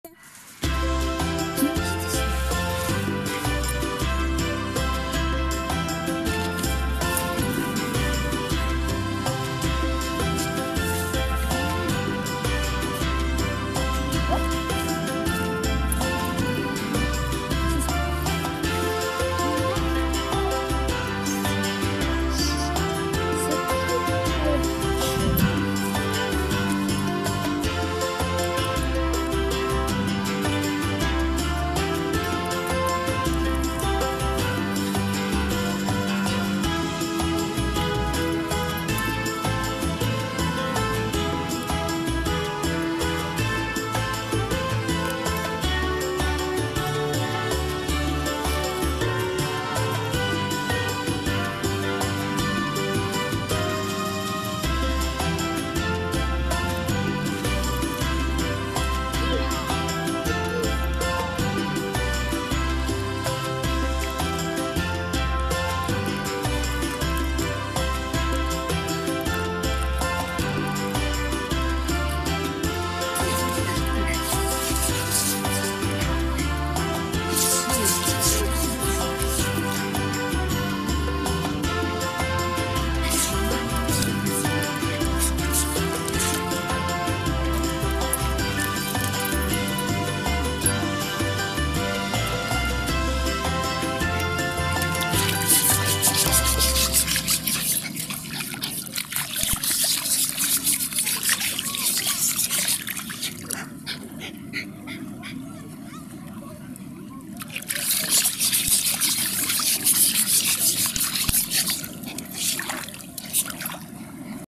Funny Jack Russell Terrier Dog Videos. Собака на каникулах! Джек рассел терьер Приколы Собаки Видео
Собака на каникулах! Джек рассел терьер Приколы Собаки. Джек рассел терьер прыгает в траве, гуляет в лесу, копает ямы на пляже. Dog on vacation. Funny Jack Russell Terrier Dog Videos / jack russell terrier funny videos. Jack Russell Terrier jumping. Dog digs a hole in the beach. Jack Russell Terrier on the beach